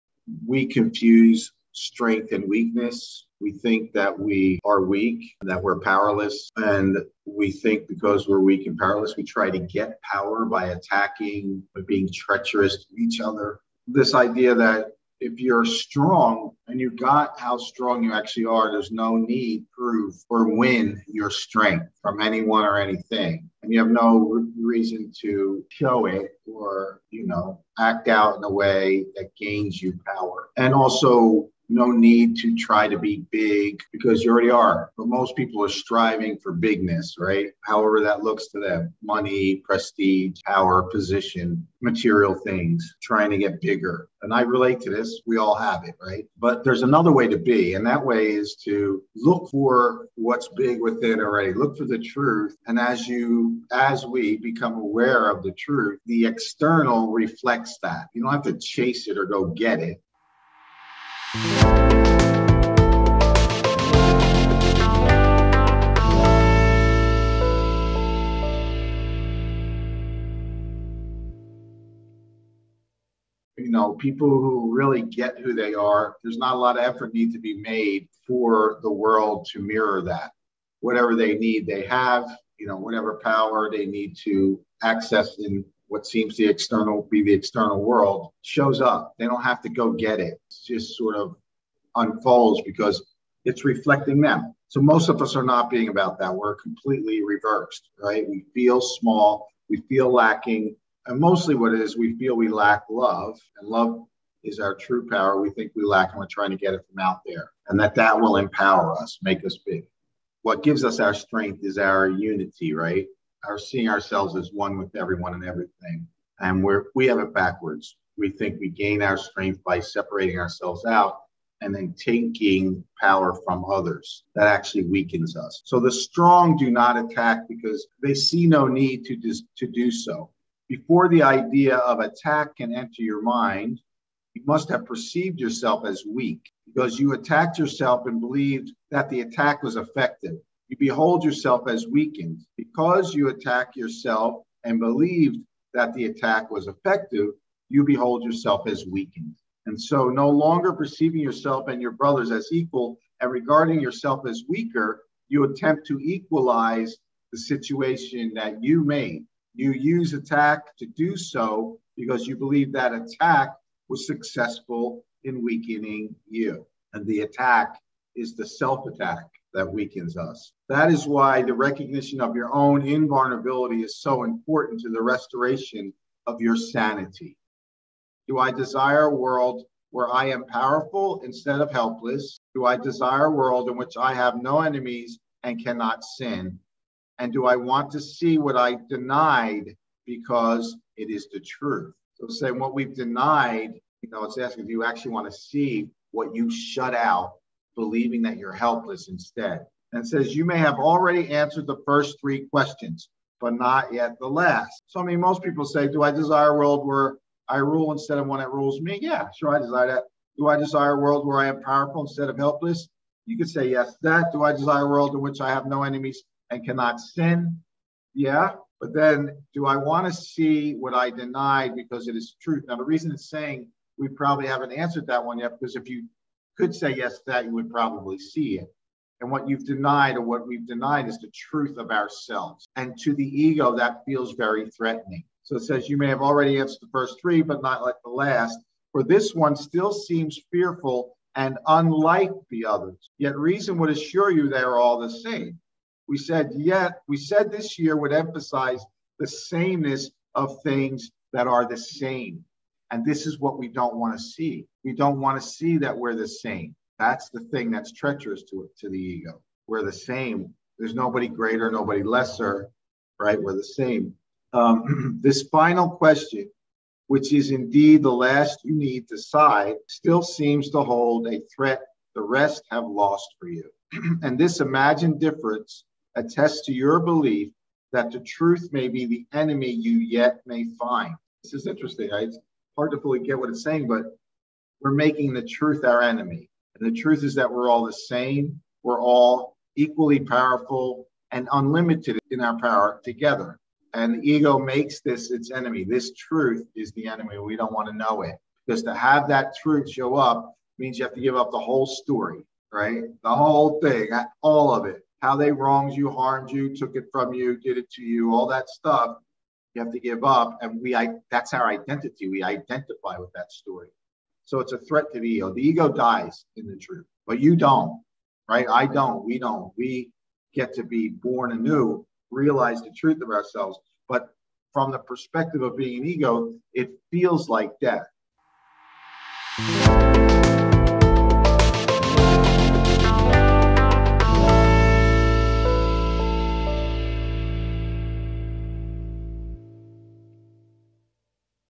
The Sane Curriculum | Miracle Makers Community with Mark Van der Gaag
A Course in Miracles Discovery Group with Mark Van der Gaag "The Radical Reboot Coach"
Get exclusive access to A Course in Miracle trainings and resources!, create or join your own Miracle Makers Vision Team, and connect with your tribe